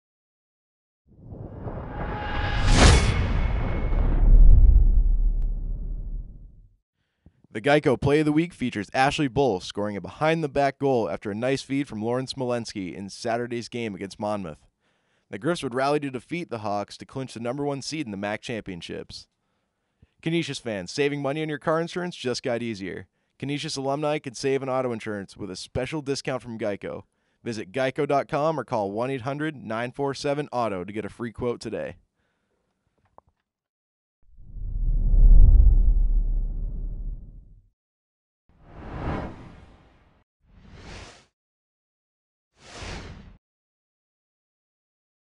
Geico Play of the of Week
This week's Geico Play of the week features a behind the back goal from Ashley Bull.